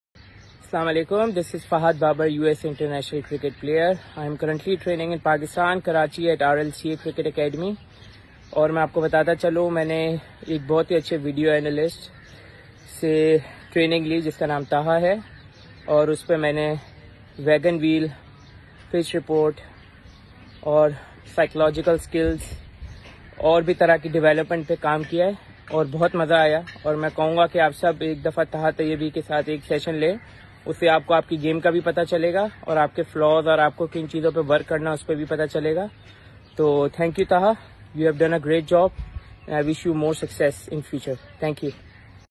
USA International Cricket Team Player Fahad Babar.
USA International Cricket Team Player
Fahad Babar .
Shares his views about Scientific Cricketing Sessions & TESA Cricket ACademy.

Contact Now : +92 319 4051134